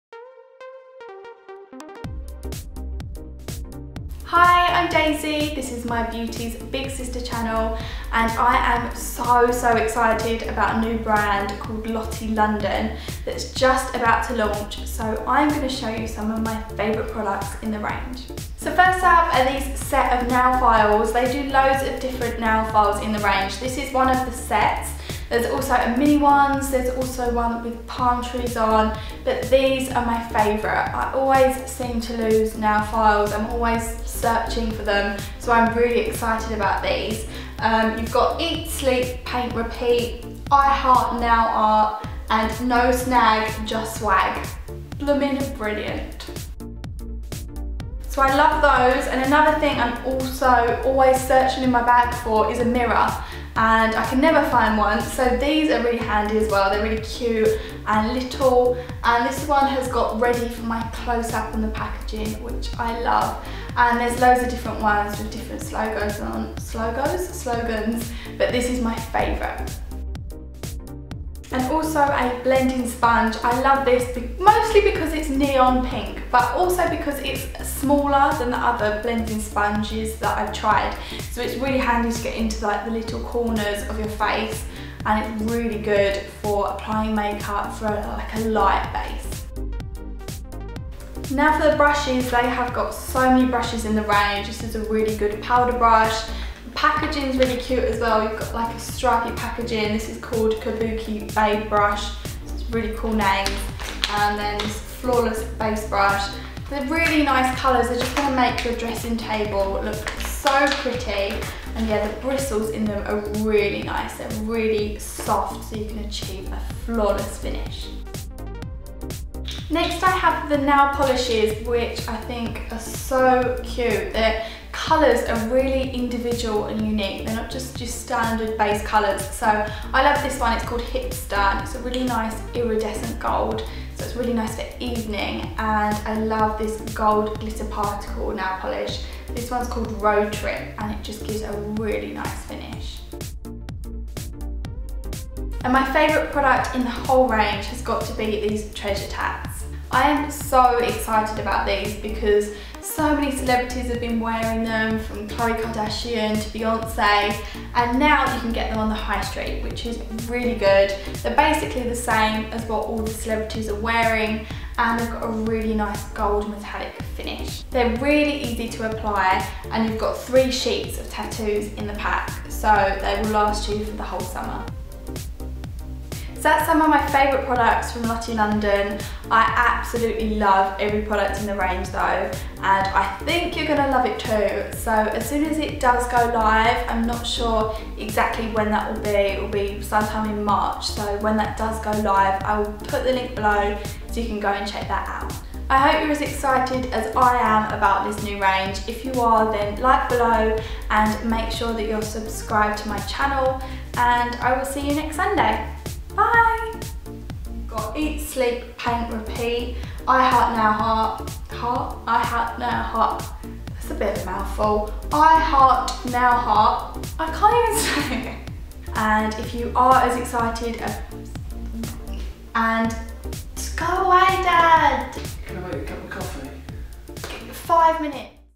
NEW BRAND! Lottie London | Beauty's Big Sister
A new brand is launching soon, and I LOVE it! Here's a sneak peek of my favourite products in this new range; Lottie London. Enjoy!
*COMPETITION*
Keep an eye out on my Twitter & Instagram for a competition to win some Lottie London products soon!